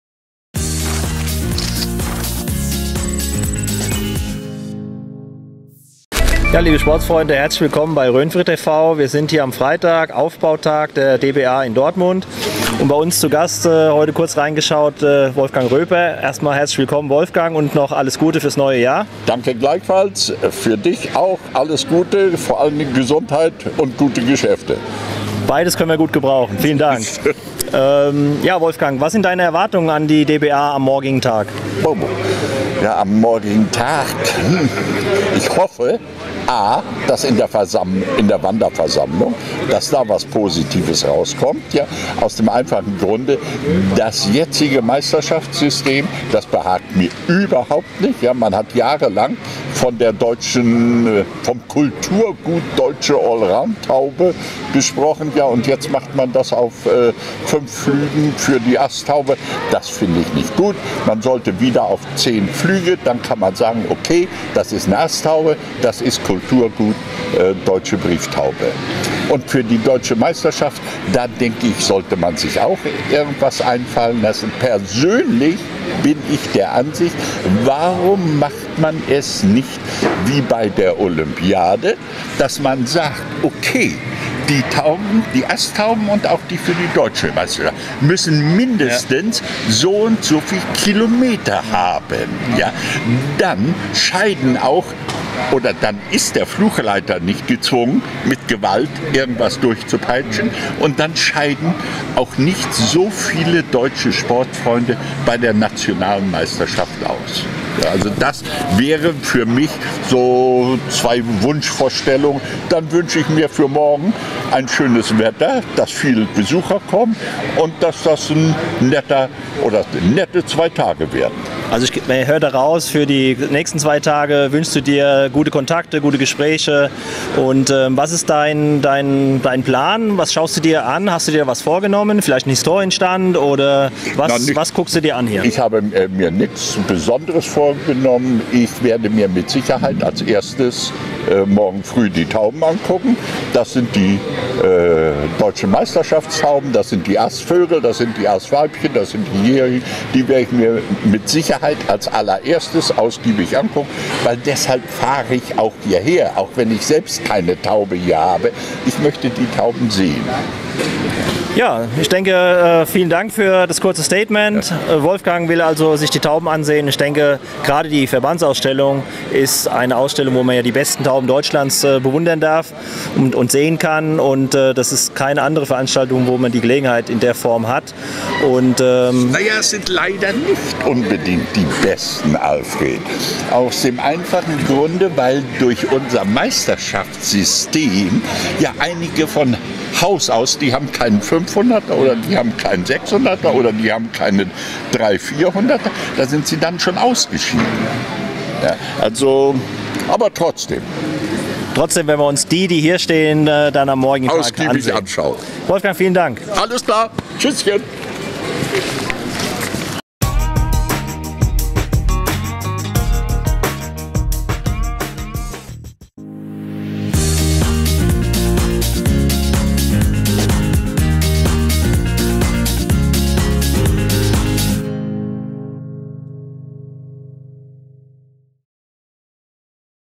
Wolfgang Roeper im Kurzinterview mit Alfred Berger beim Aufbau der DBA 2016 (Brieftauben)
Wolfgang Roeper im Kurzinterview mit Alfred Berger beim Aufbau der DBA 2016